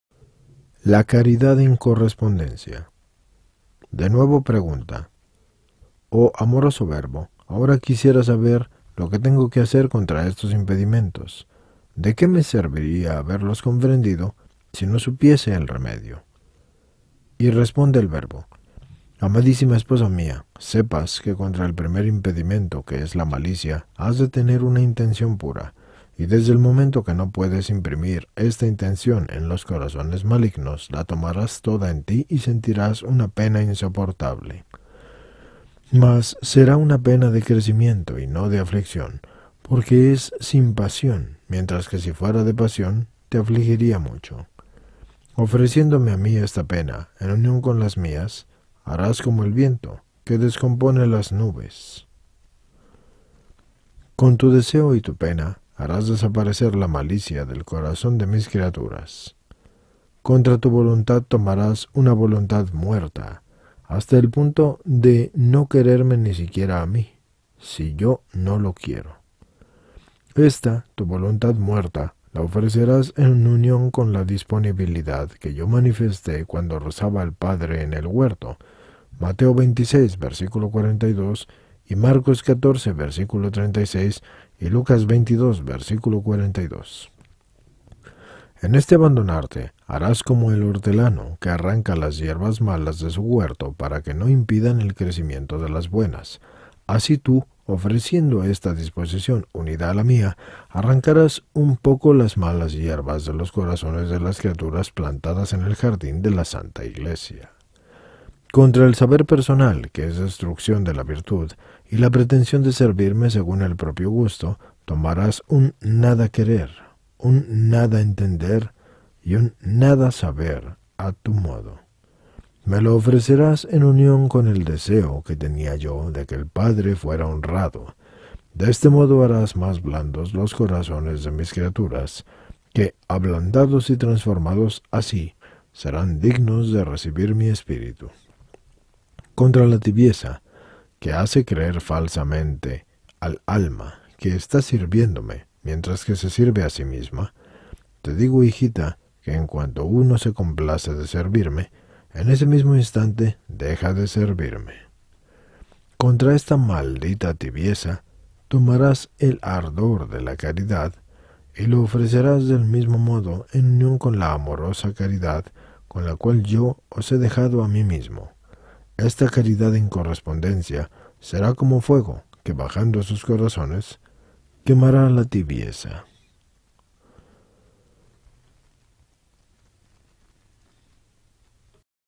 36 REVELACIONES E INTELIGENCIAS PRIMERA NOCHE 7 La Caridad en correspondencia
Libro "Éxtasis, Amor y Renovación" Santa Magdalena Pazzi

Éxtasis, amor y renovación. Con estas tres palabras definimos la vida y escritos de santa María Magdalena de Pazzi (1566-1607), gloria del Monasterio degli Angeli, en Florencia, donde se conservan la reliquia de su cuerpo y los manuscritos de los cinco libros de experiencia sobrenatural. En este volumen se ofrecen dos de sus obras: «Libro delle Renovationes e Inteligentie» y el «Libro della Renovazione della Chiesa». Con mucha riqueza extática como en esta santa. De su experiencia mística reflejada en sus escritos resulta una cristología de pura identificación con Cristo en su pasión y muerte. «Padecer, y no morir», dice Magdalena de Pazzi comentando el «padecer o morir» de santa Teresa. Por su Renovación de la Iglesia parece un eco de santa Catalina de Siena.